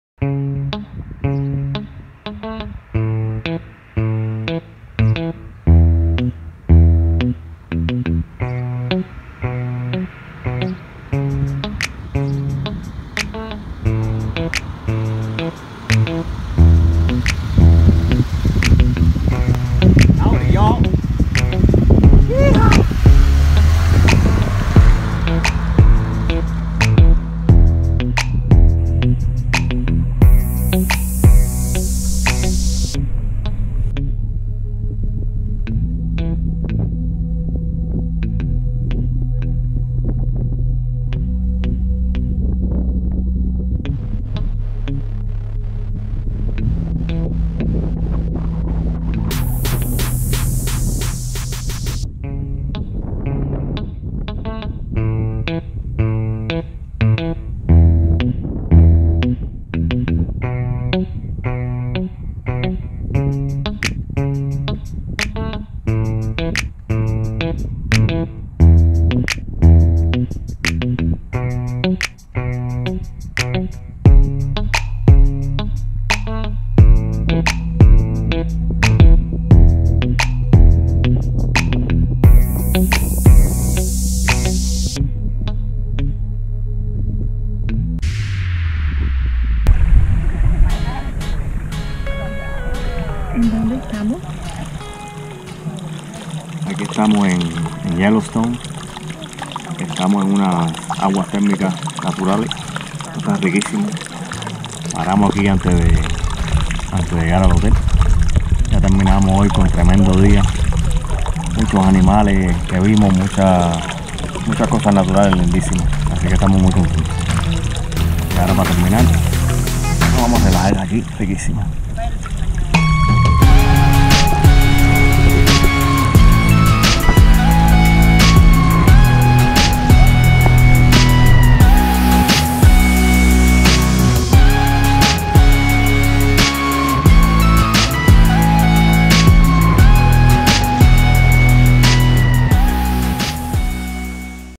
Final del viaje a Yellowstone
Asi terminamos nuestro recorrido por todo el parque nacional Yellowstone en Wyoming. Pero nuestro viaje en carretera sigue hacia Montana, no se pierdan esos videos por venir.